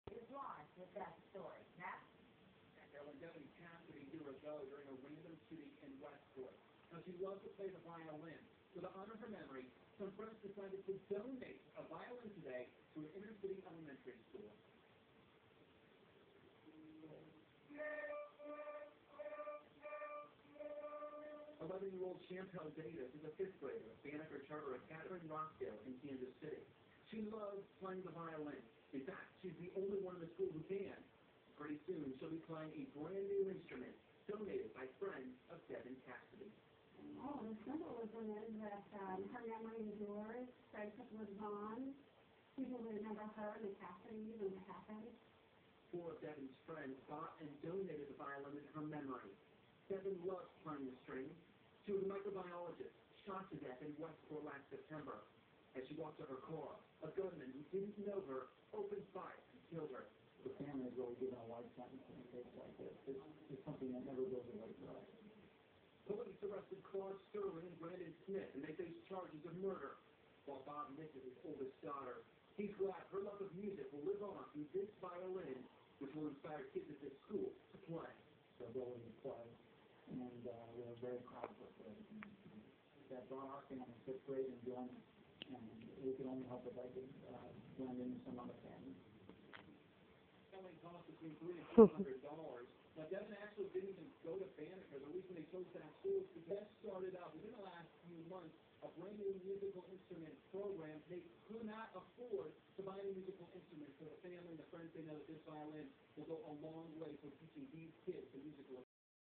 Affectionate Lovebird
Pina getting a massage